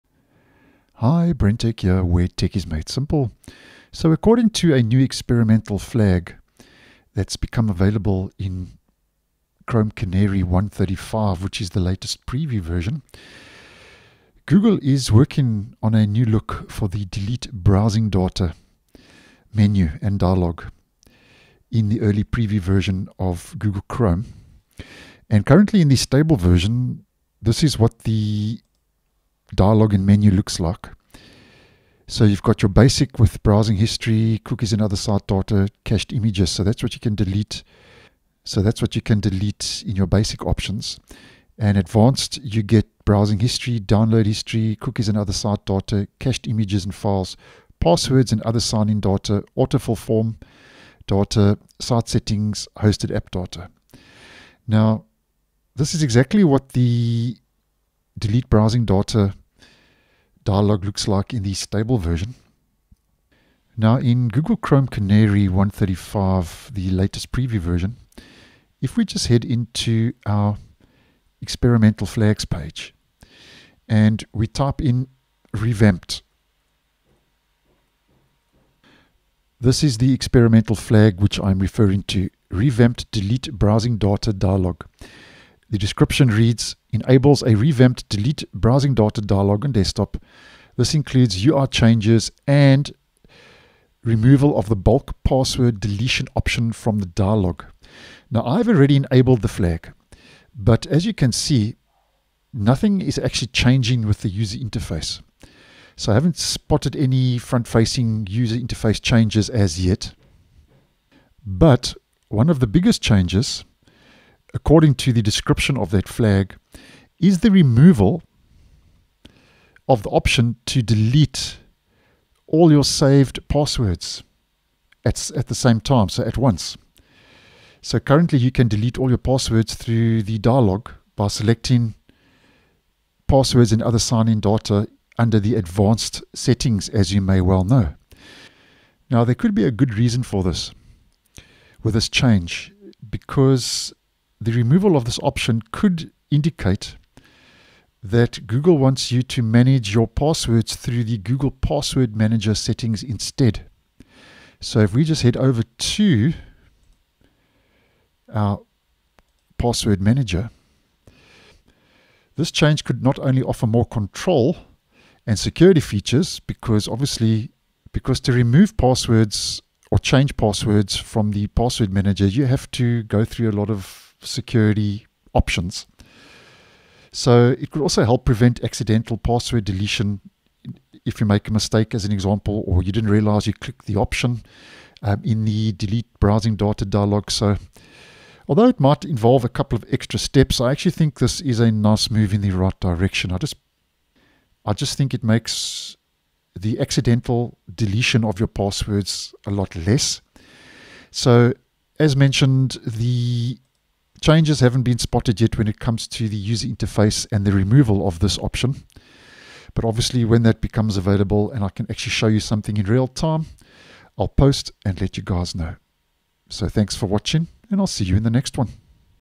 No More Password Deletion Accidents! Chrome's New 'Delete Browsing Data' Dialog
The new Delete Browsing Data dialog UI changes are currently being tested behind a flag in Chrome Canary 135